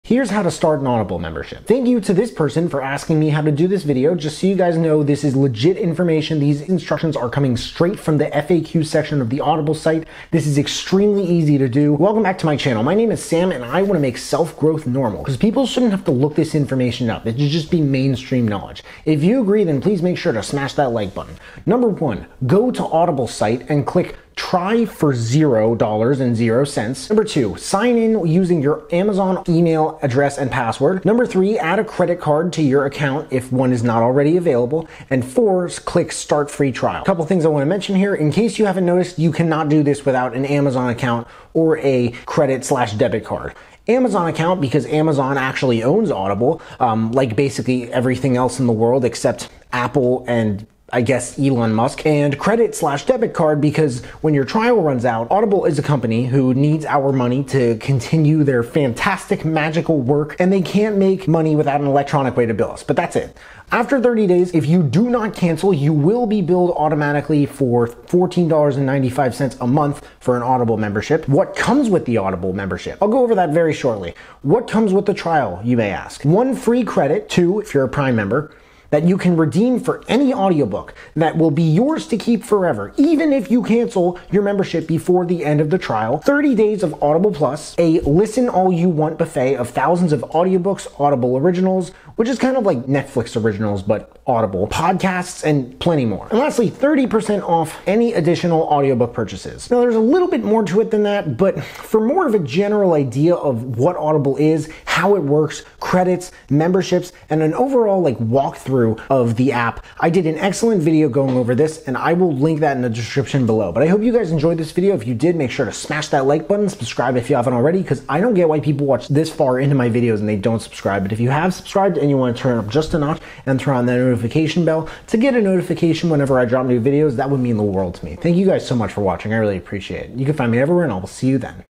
How to Start an Audible Membership | Audiobook App Tutorial
Here’s how to start an Audible membership. This is an audiobook app tutorial for Amazon and the membership process has now been explained! Just so you guys know this is legit information, these instructions are coming straight from the FAQ section of the Audible site.

40 OF MY FAVORITE BOOKS 📚 (And WHY I Think You Should Read Them!): 🤓